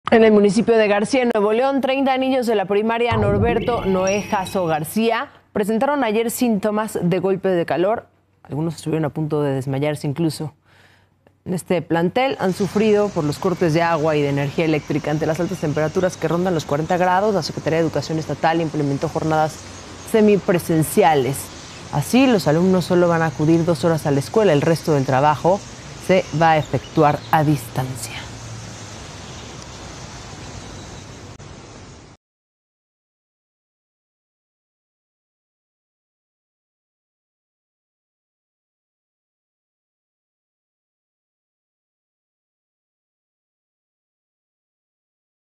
Ola de calor: Alumnos de primaria a punto de desmayarse, Nuevo León - Paralelo 23
Al menos 30 estudiantes de una escuela primaria de García, Nuevo León, manifestaron síntomas de golpe de calor y estuvieron a punto de desvanecerse. Las temperaturas en esa zona rondan los 40 grados